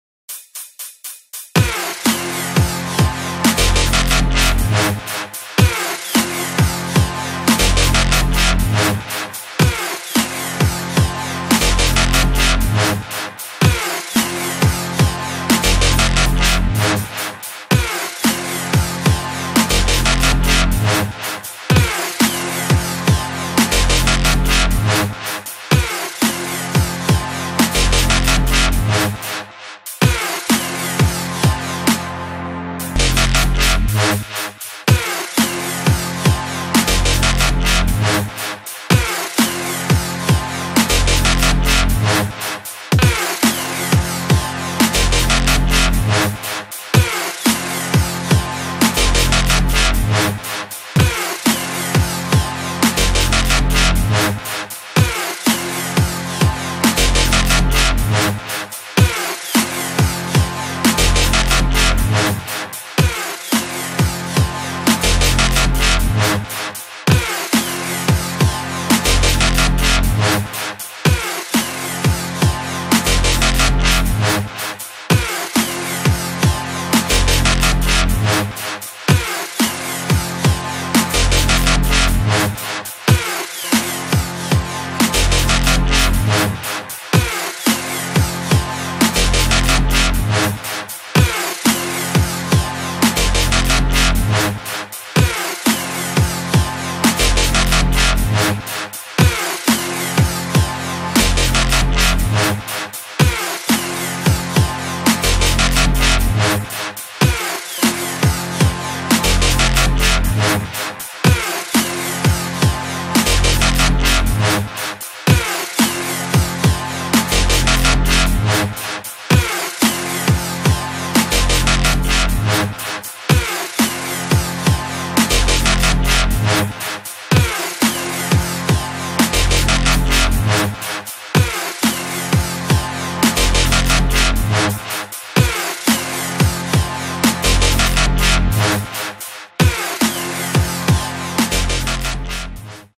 DJ Leggs hard thugging
Newfound Joy of being a DJ this is something I've been doing I will be making a separate YouTube channel for this please enjoy music  by DJ Leggs